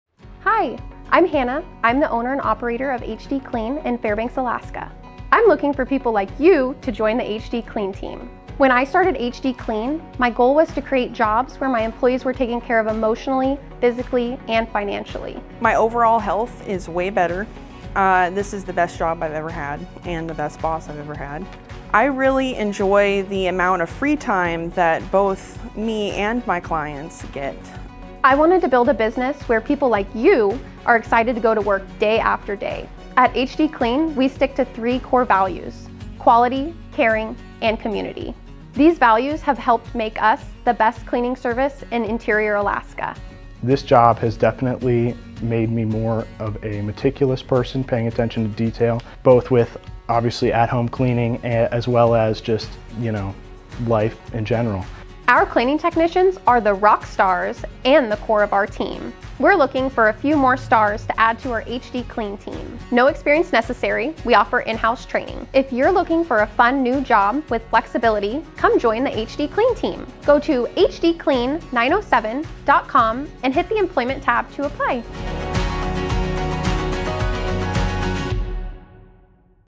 HD Clean Recruitment Video
Hannah, Megan and Jack tell us about working at HD Clean and why it's a great choice for anyone looking for work!